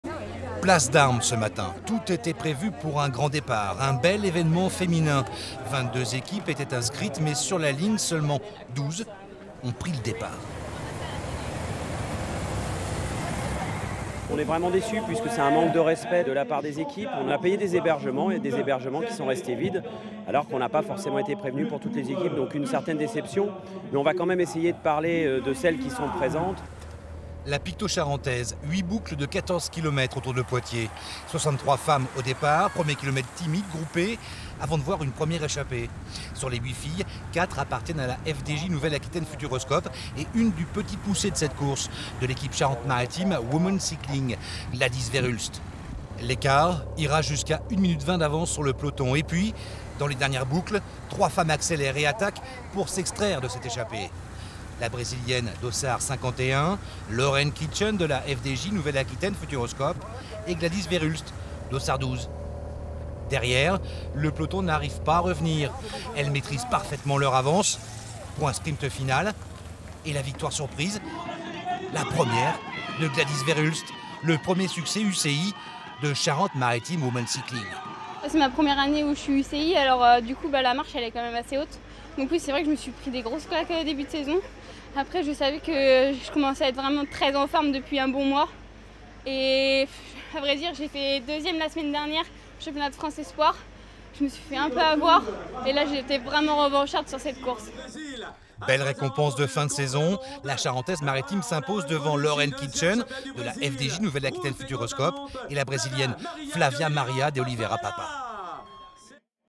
Gladys Verhulst remporte la Picto-Charentaise : résumé de la course
Poitiers : Gladys Verhulst (Charente-Maritime Women Cycling) remporte la Picto-Charentaise

Reportage d'Olivier Riou